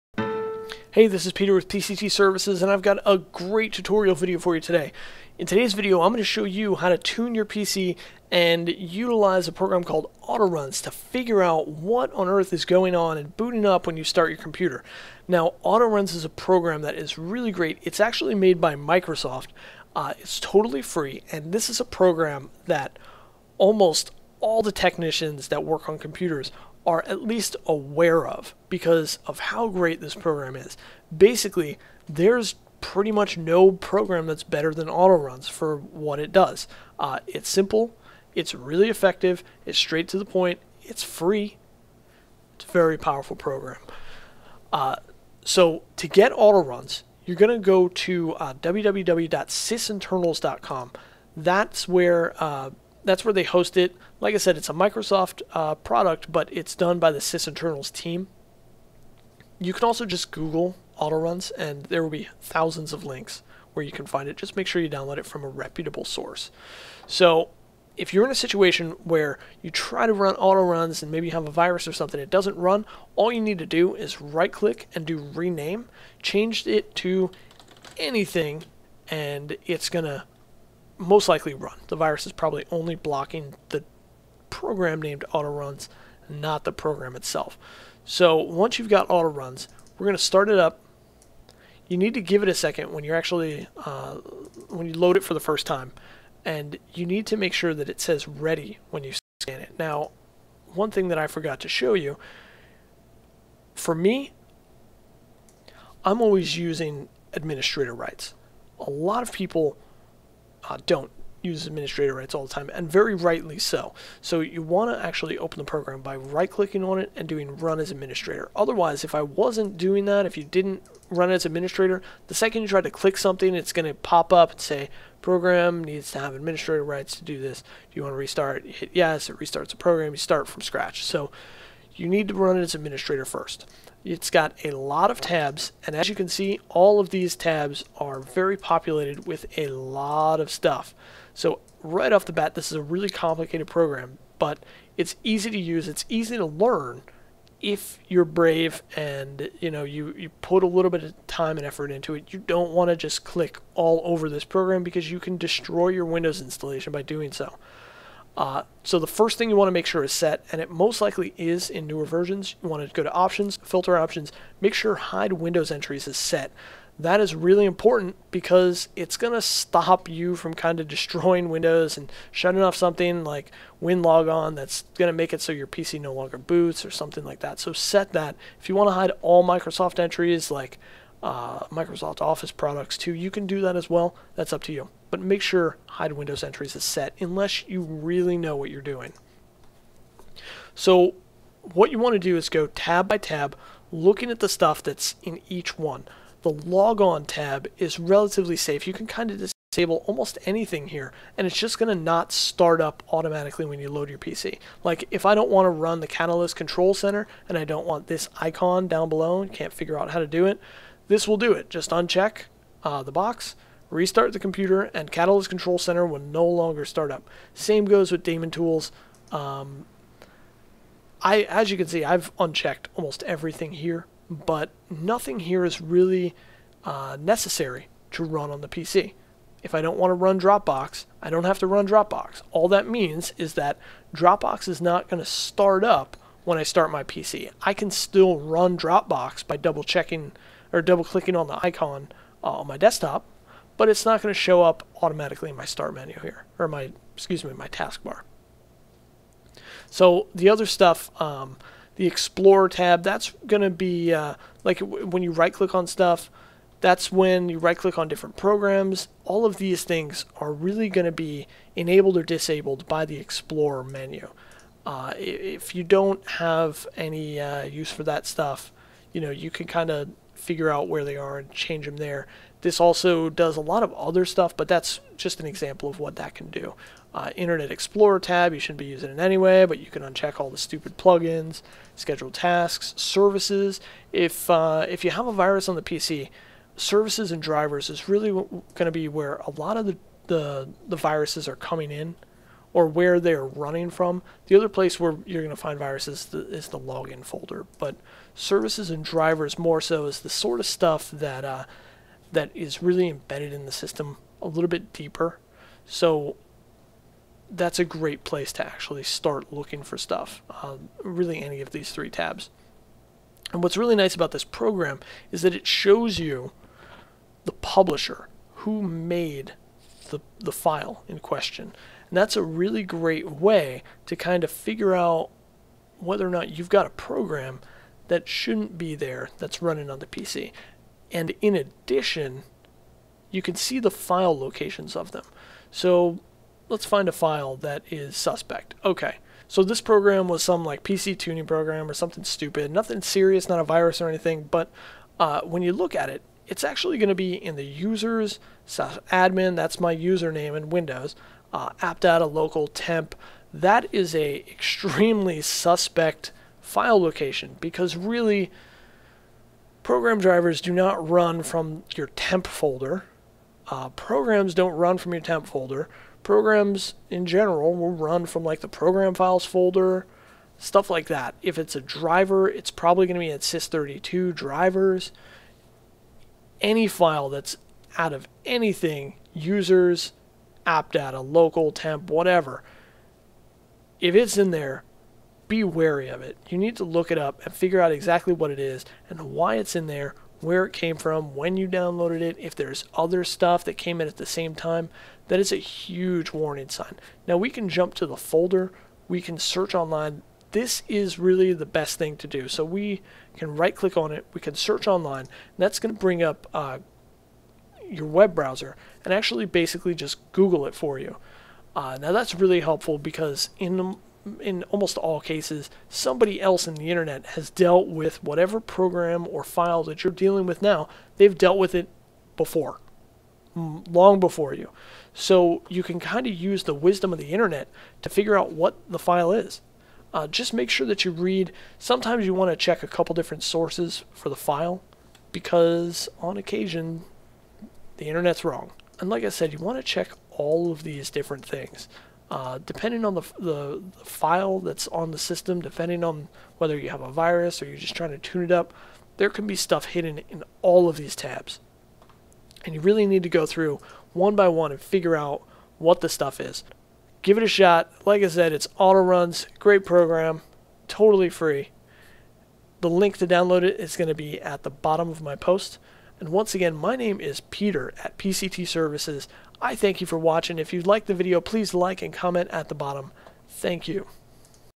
Using Autoruns to optimize your computer & find viruses
Peter @ PC-T Services here with a great tutorial on Autoruns. Using this free Microsoft program you can tune your PC to speed up boot time. In addition you can use Autoruns to locate and identify rogue software that may be running on your PC.

Peter Downey
PC-T Services